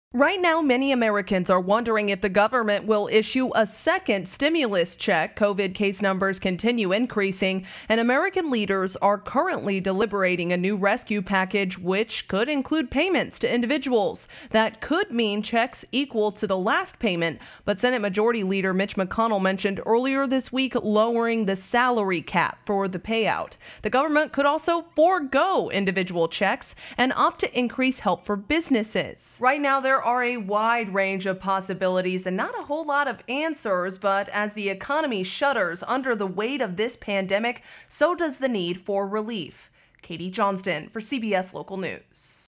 What A Second Stimulus Payment Could Look Like
A second stimulus payment grows more likely by the day, with coronavirus surging and the fate of the economy hanging in the balance. Katie Johnston reports.